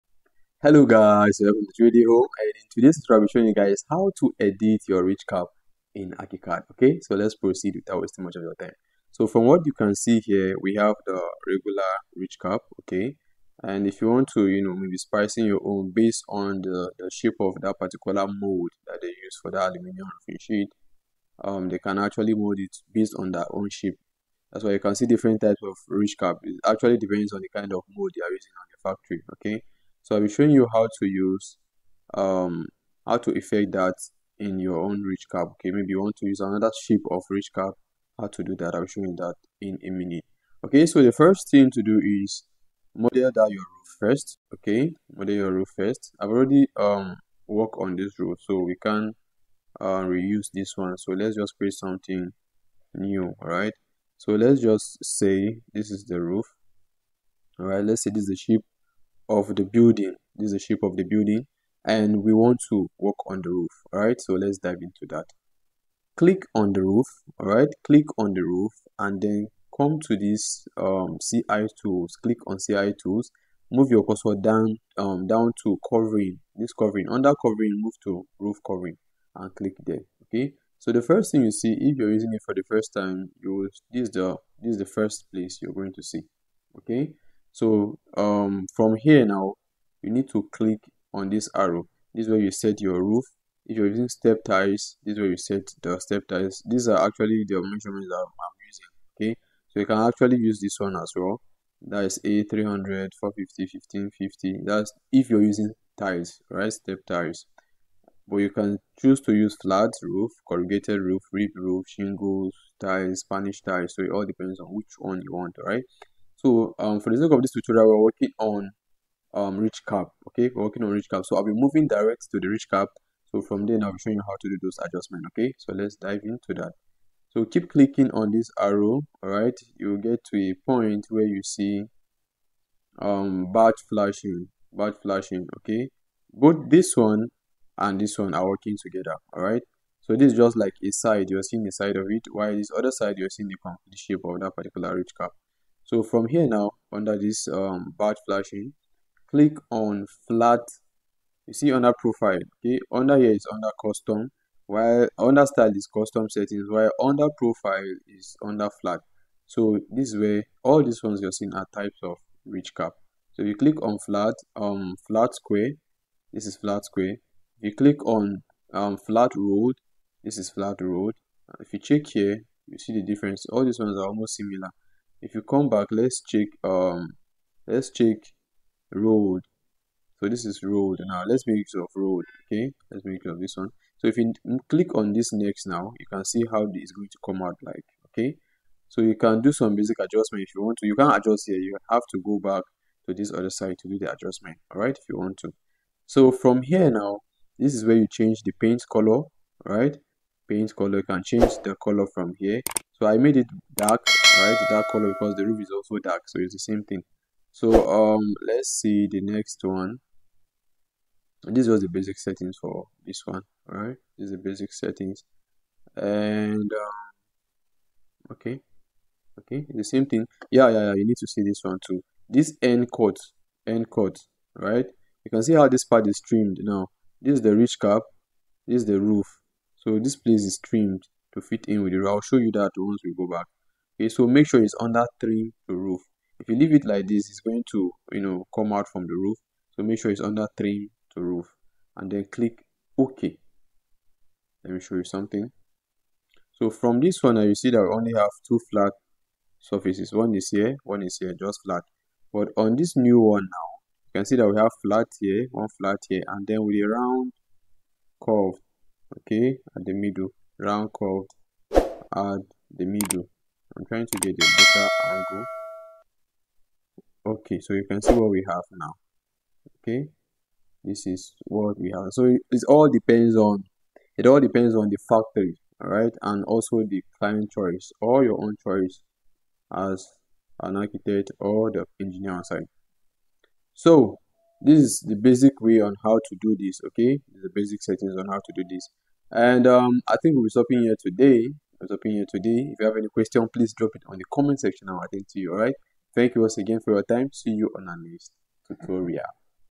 Ridge Cap in Archicad
In this Archicad tutorial, you will be learning how to assign various types of ridge Cap to your roof in Archicad